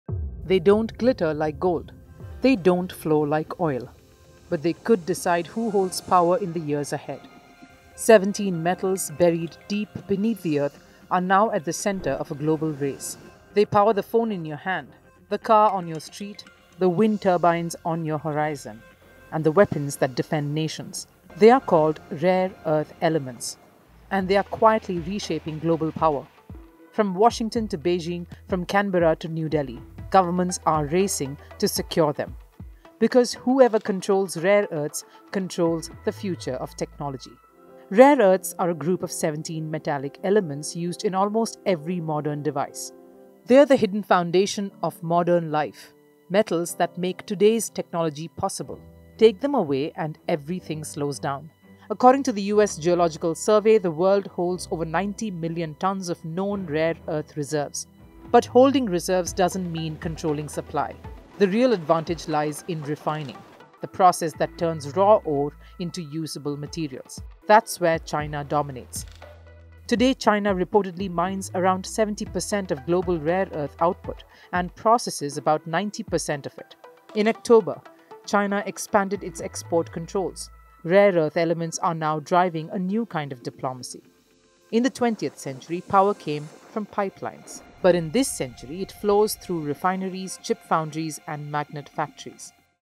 What Are Rare Earths: The Gold Everyone Wants? | Firstpost Tech & Trade | N18G
Rare earths are seventeen metals buried deep beneath the Earth that power the modern world. From smartphones and electric cars to wind turbines and defence systems, they are the invisible backbone of today’s technology. But control over them is uneven. China dominates mining and refining, giving it leverage over global supply chains. Now, the United States, India, Japan, and Europe are racing to secure alternatives. So, what exactly are rare earths, why are they so important, and how could they shape the next era of global power? Watch this video to know more.